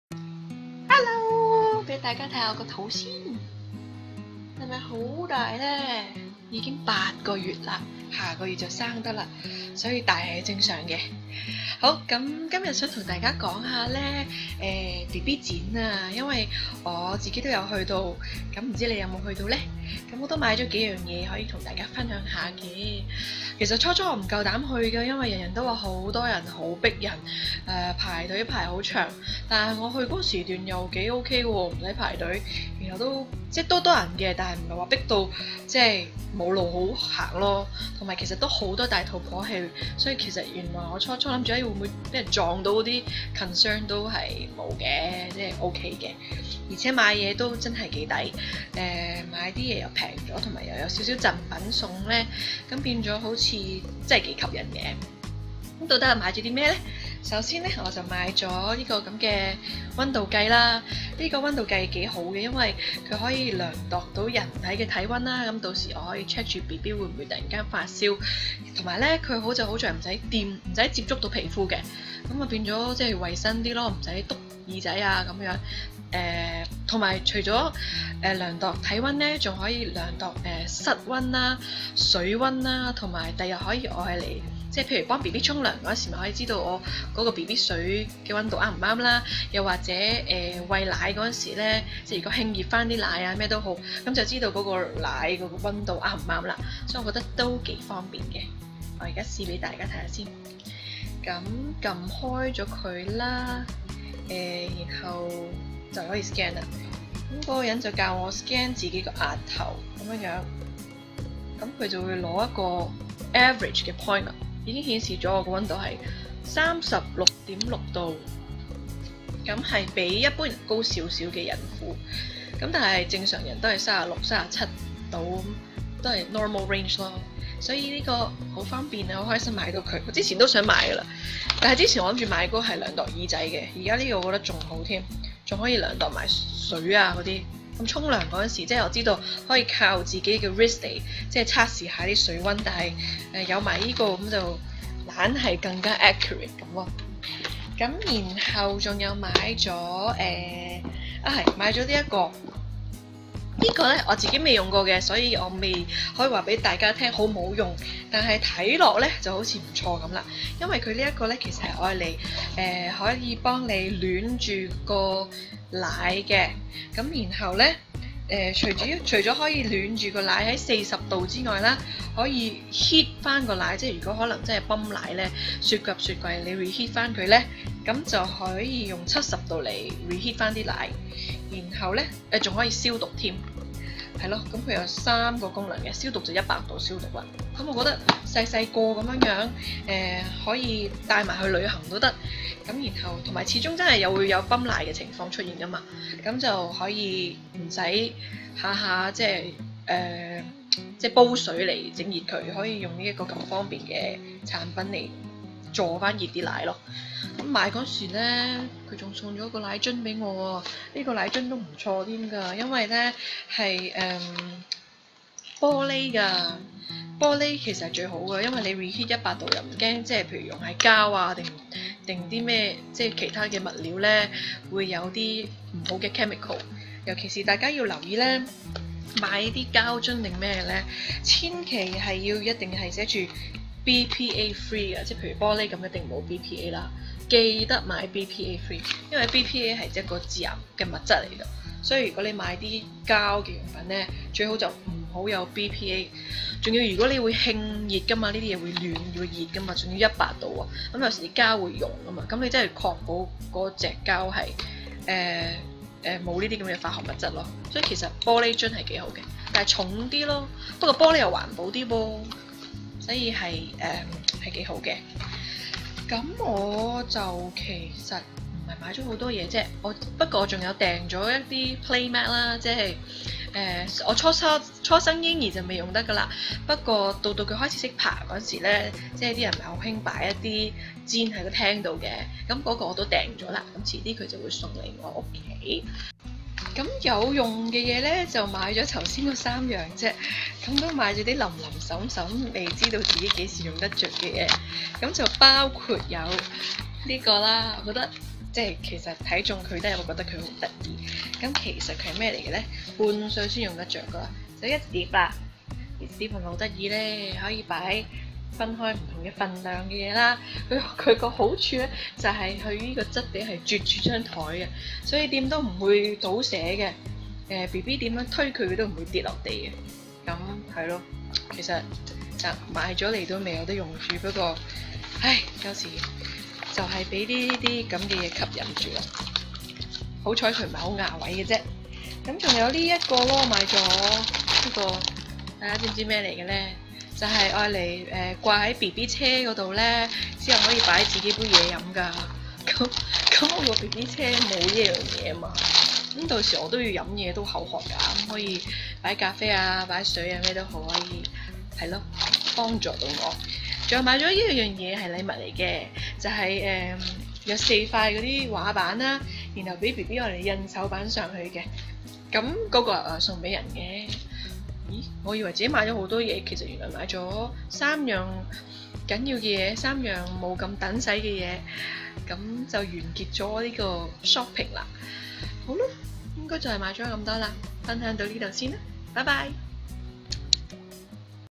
又買左多幾樣BB用品! Do you need these baby items!?
Welcome to Mama College!
上星期去左嬰兒用品、兒童用品展覽，都幾多東西買。
分享一下我又買左咩有用或沒用嘅BB野!

misskayicheung

Kayi Cheung
Mama College